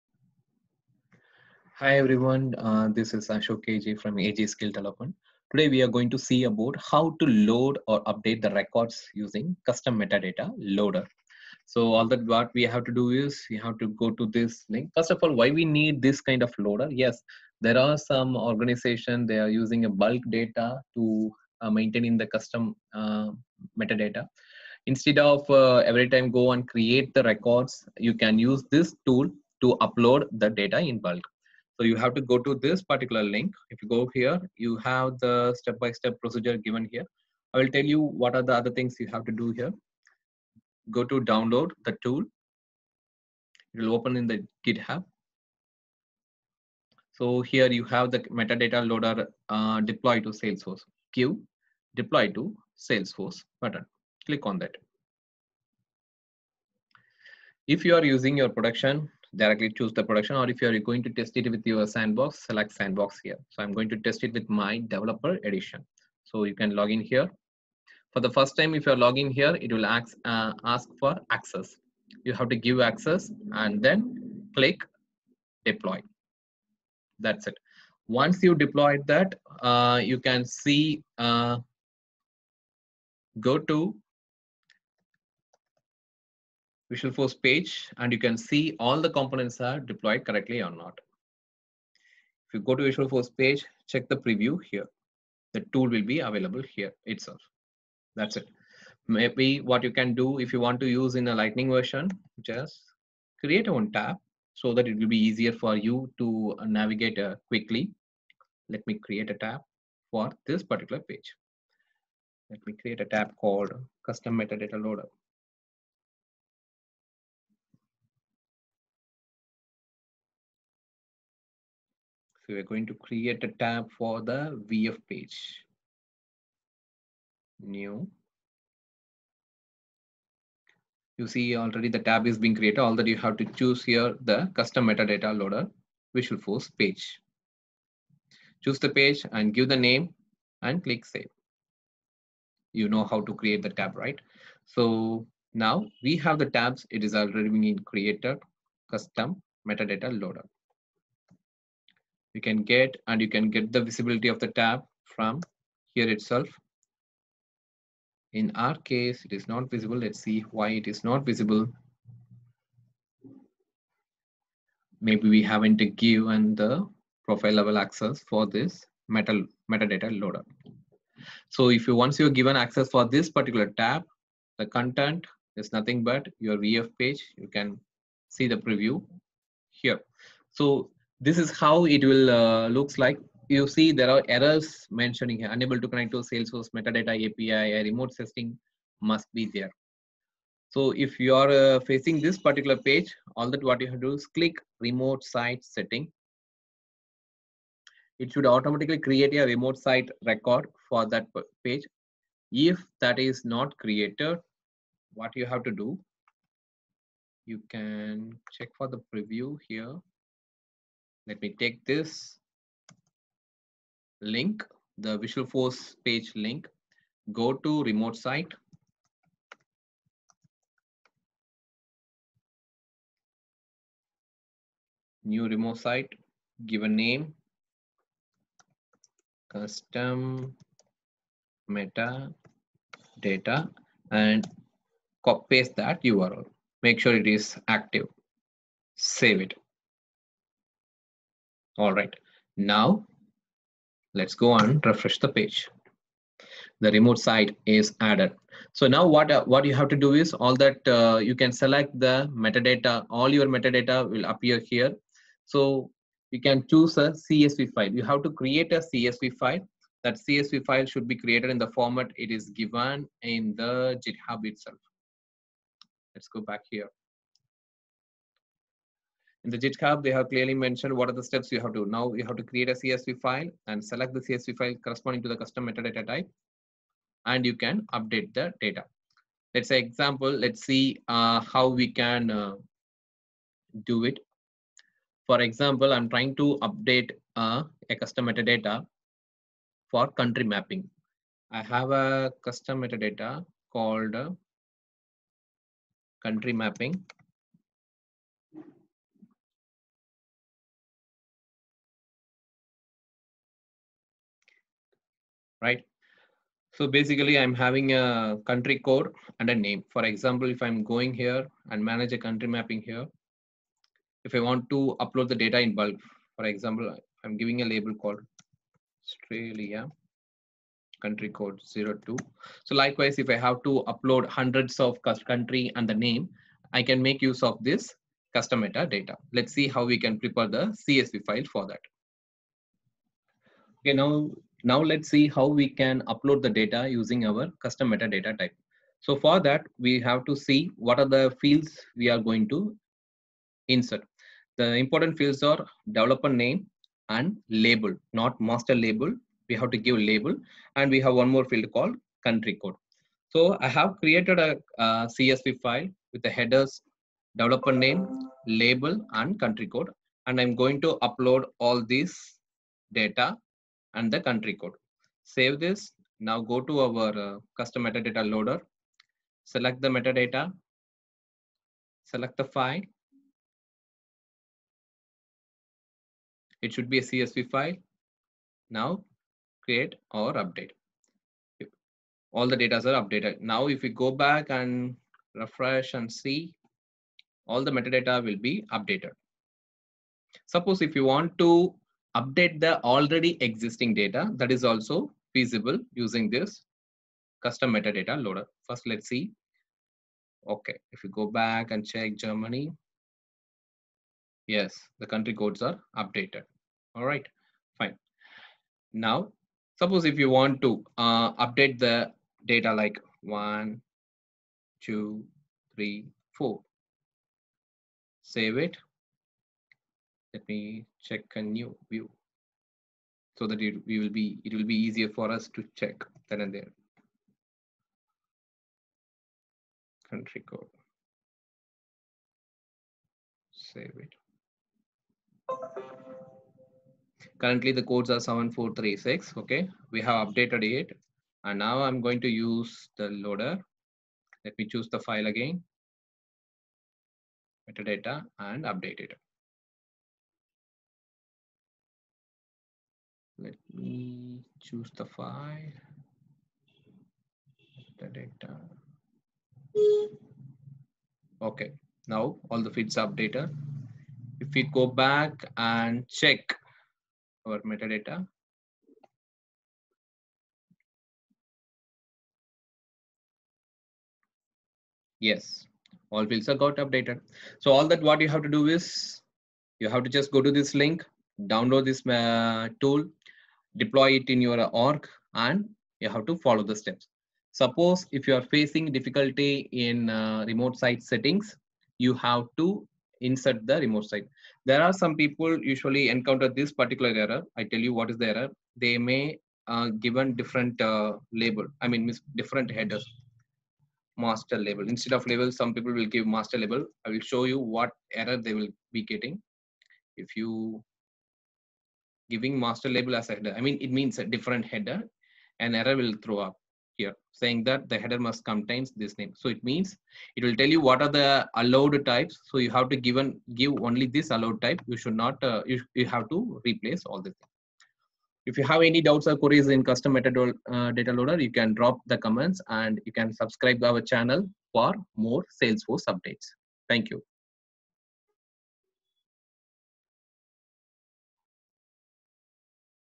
Custom Metadata Loader to bulk load or update records of custom metadata types -AJ Skill Development
Load or Update Records with the Custom Metadata Loader